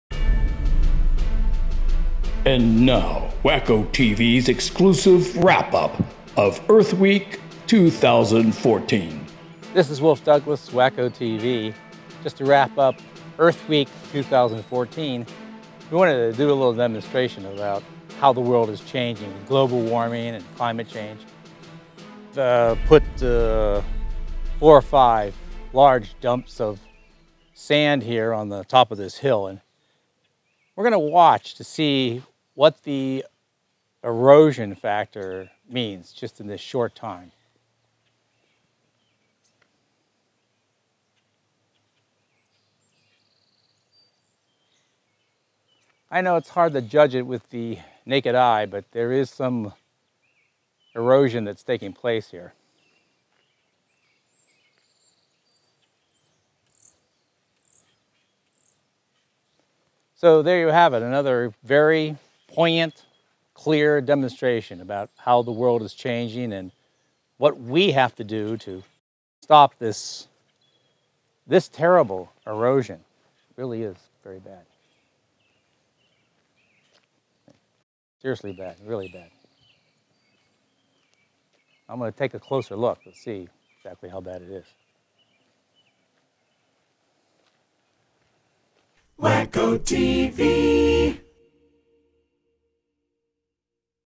WHACKO-TV EARTH WEEK 2014
Wow, we are so pumped up here at WHACKO-TV. To celebrate Earth Week 2014, we have been recycling some old bits from the 50s and 60s. And we have been only eating biodegradable food. It's been fun seeing how the whole staff (and the rod) pulled together to honor the earth in such a nice way. Wolf Douglas even cooked up this demonstration to prove how doomed we are as a planet. Do I smell EMMY? Are those biodegradable?